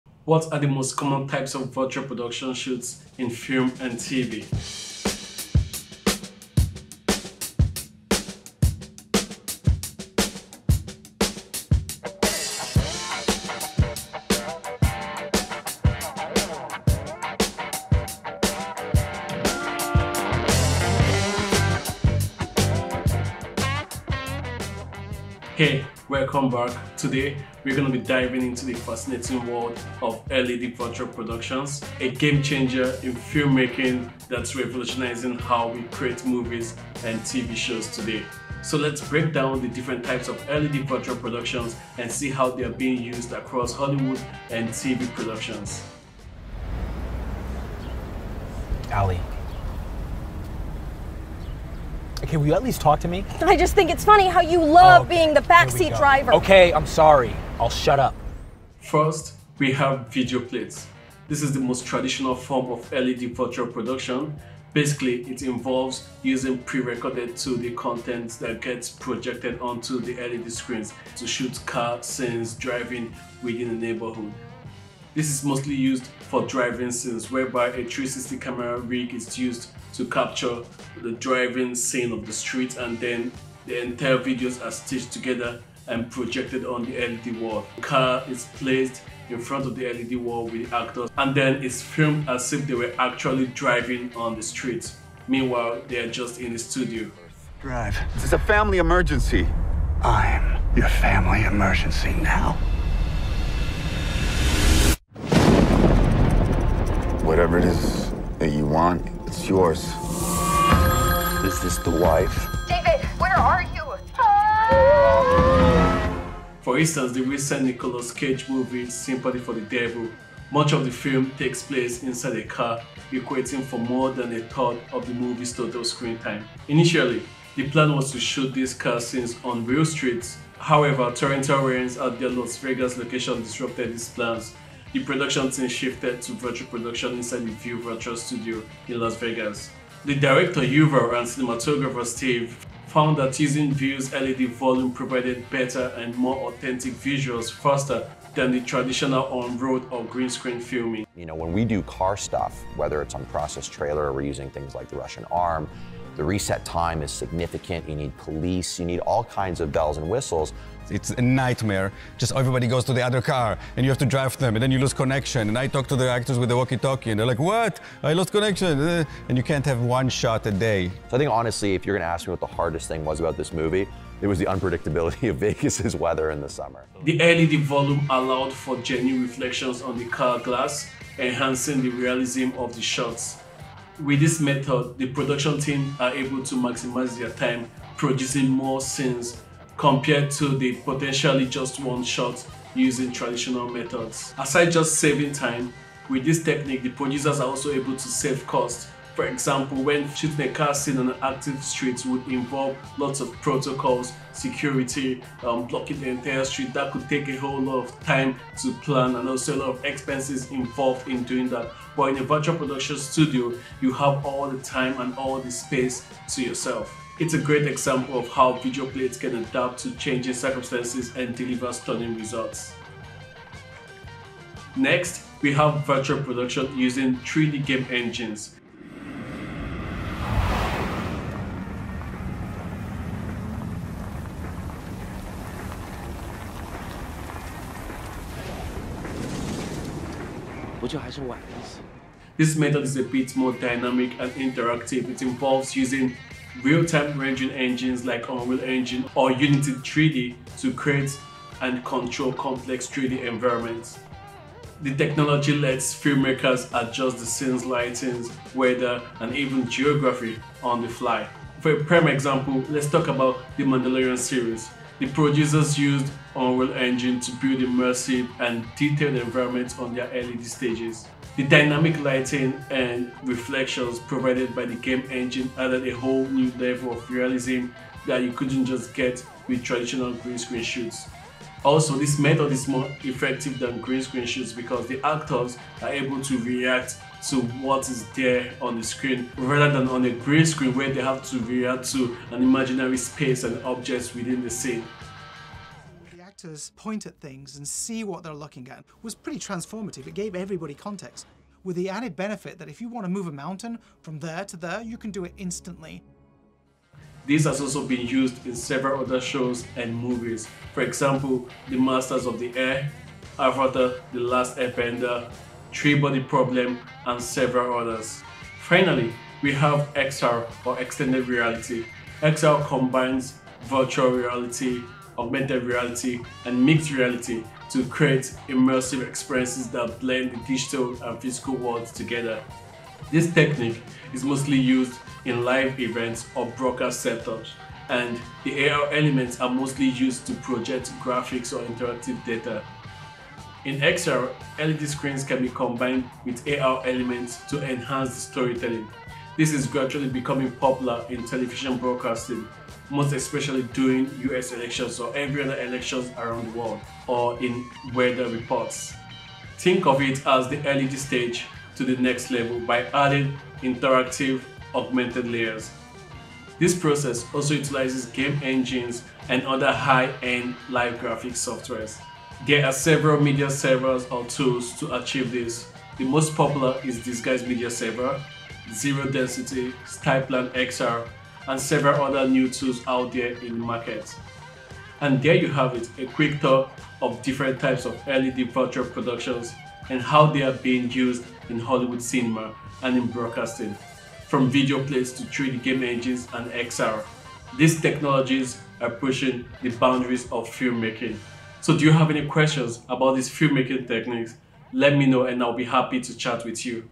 Types of LED Virtual Production: From 2D Backplates to XR Innovations
What are the different types of LED virtual productions? Here, I've outlined some of the various virtual production techniques used in film and broadcast, ranging from 2D backplates to 3D environments and extended reality (XR) setups.